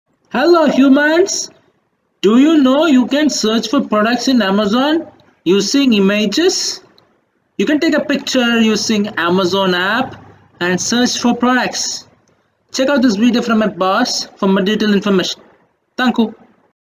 Search for products in Amazon using images
Use Amazon app to search for products by images.